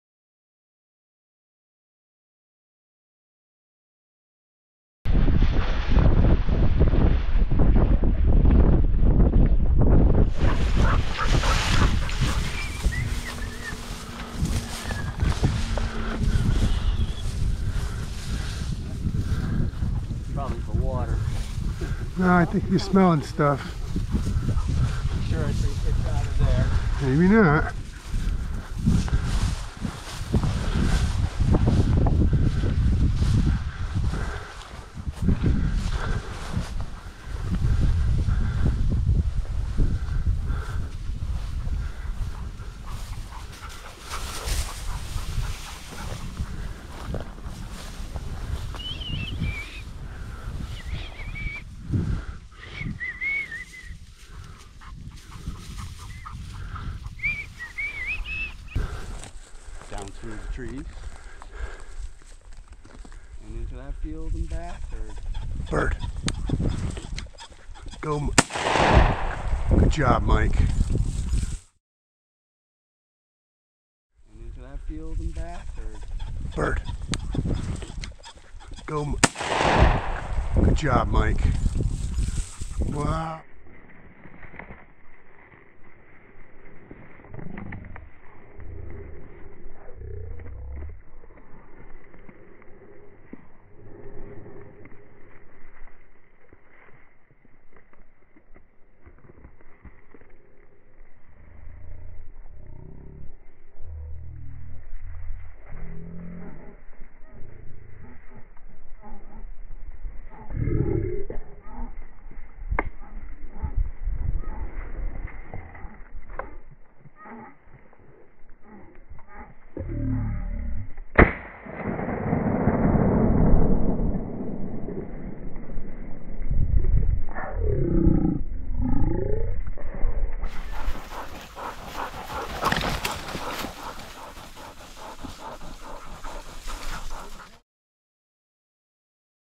A Day in the Fields w Augie and Clancy
Augie and Clancy on a hunt. Springer Spaniels Hunting